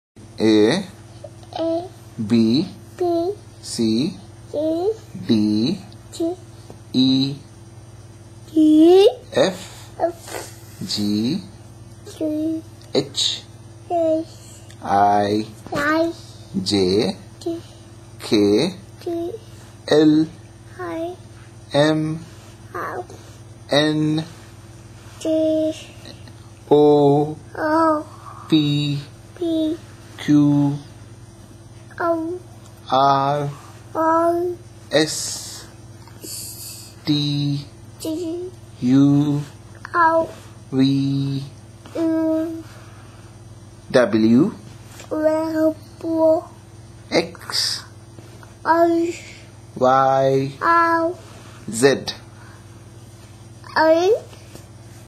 Ishaan learning to speak
Ishaan can spell ABCD now.